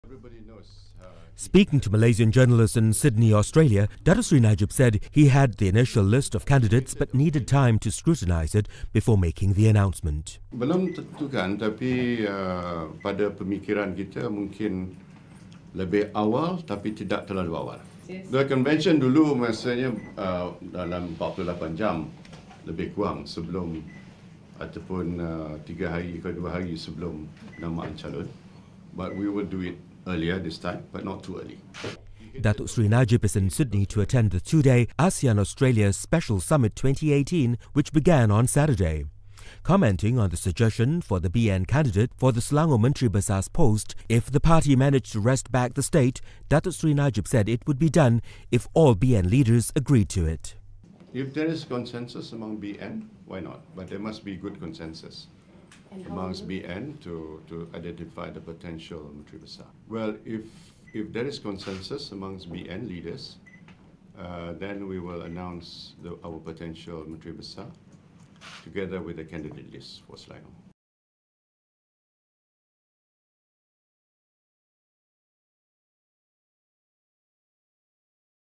GE14 - BN May Announce Candidates Earlier, Says PM
Barisan Nasional may announce its candidates for the 14th General Election earlier than the previous practice of announcing it two or three days before nomination day.

Prime Minister Datuk Seri Mohd Najib Tun Razak, who is also BN Chairman, said this would allow BN to solve problems in certain constituencies and for the candidates to make early preparations.